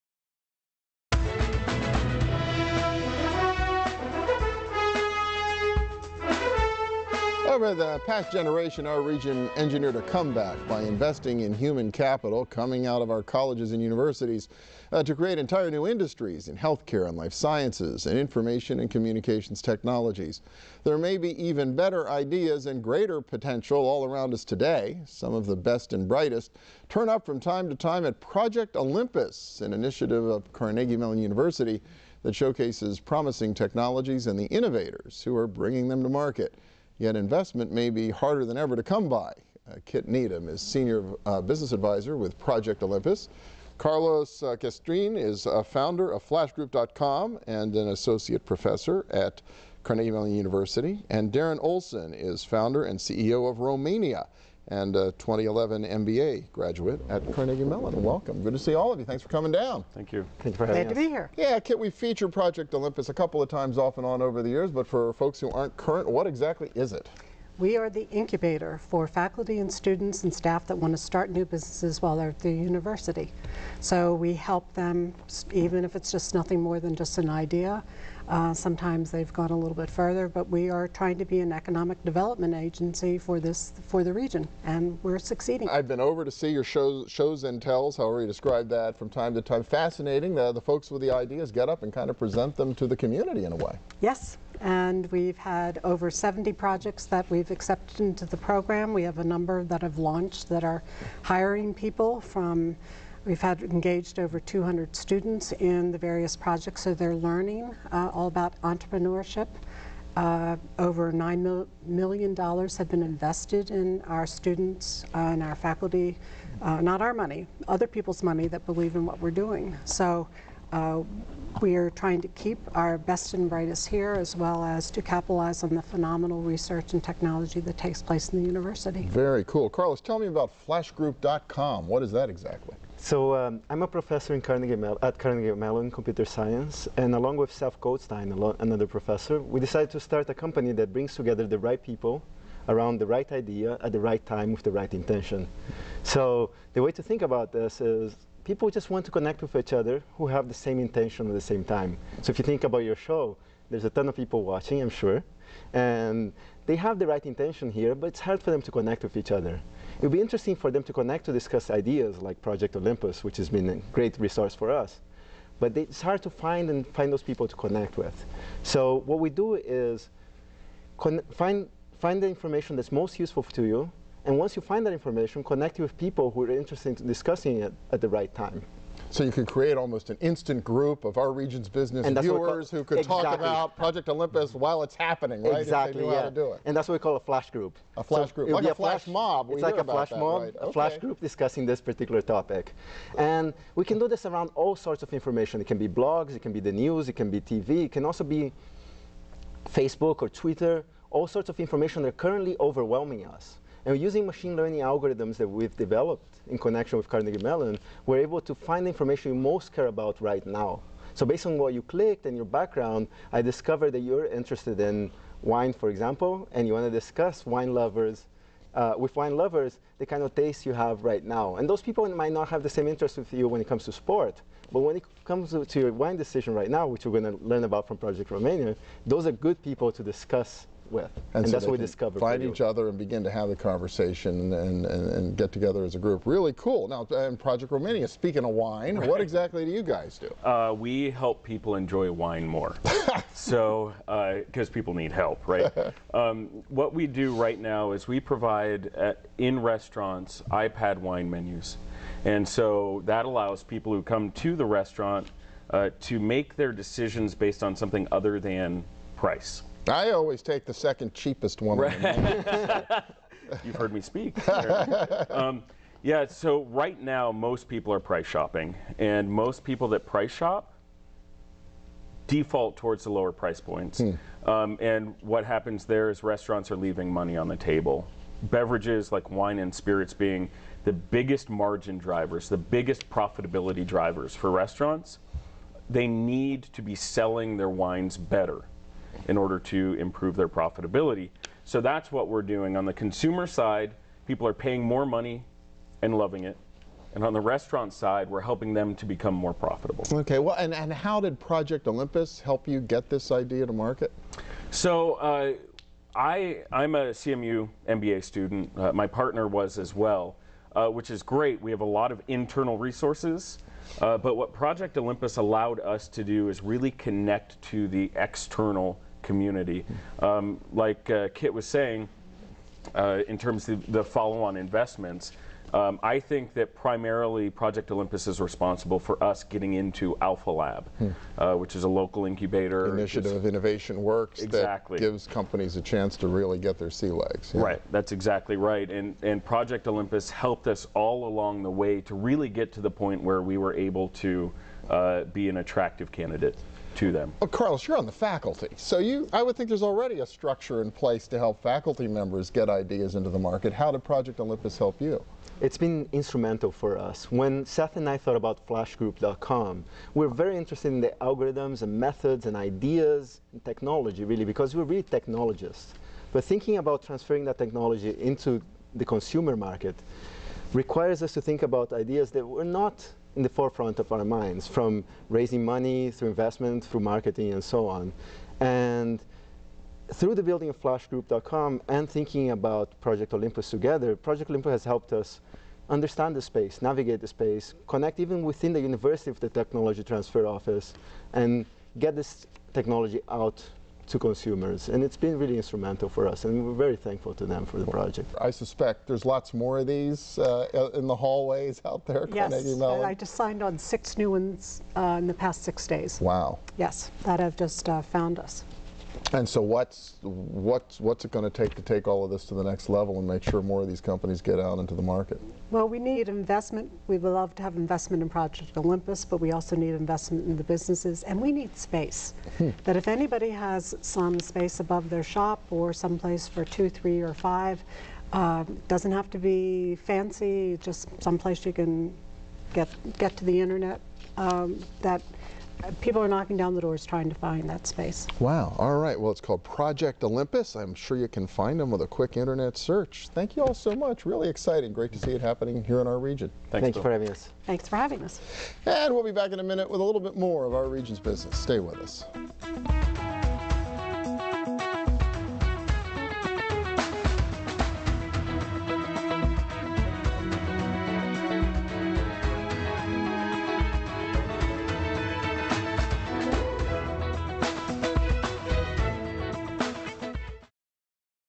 Project Olympus: Fostering the Next Generation of Tech Innovators
Over the past generation our region engineered a comeback by investing in human capital coming out of our colleges and universities to create entire new industries in health care and life sciences and information and communications technologies. Some of the best and brightest turn up from time to time at Project Olympus, an initiative of Carnegie Mellon University that showcases promising technologies and the innovators who are bringing them to market.